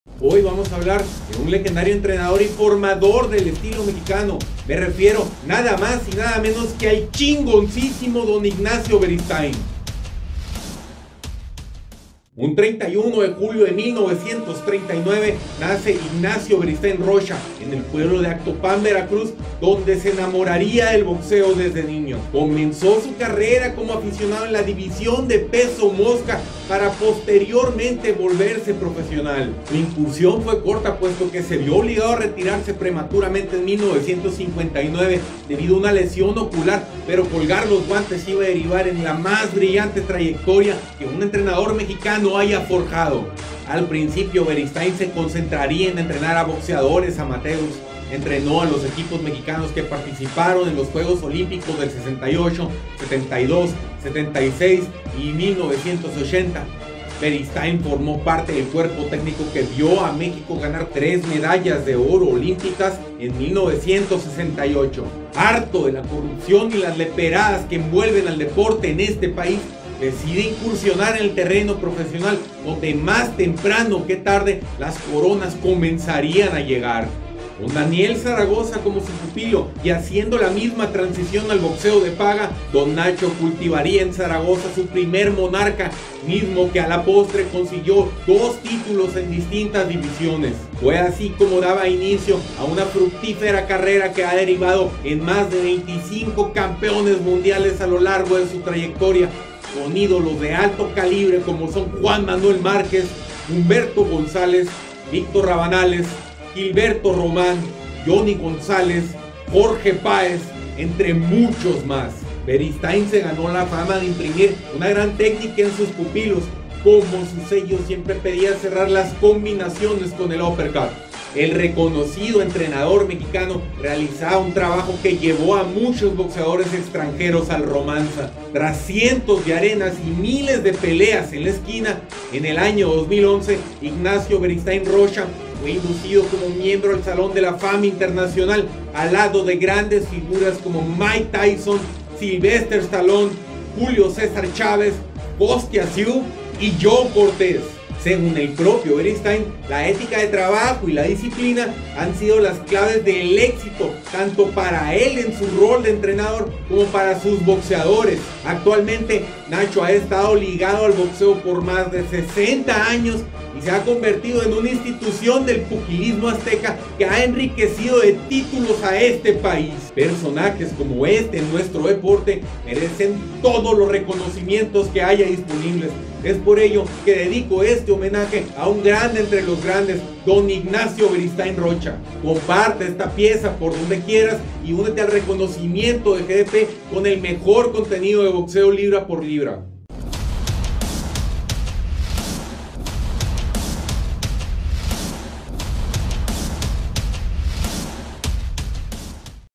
Homenaje a Nacho Beristain
Ignacio Beristáin Rocha tiene las credenciales para ser considerado el mejor entrenador mexicano que haya existido y es tiempo de homenajearlo con una pieza a la altura de su carrera.

Chécate todo lo que no sabías sobre la trayectoria de Don Nacho y rindele tributo a este orgullo del boxeo azteca regando por todos lados esta pieza.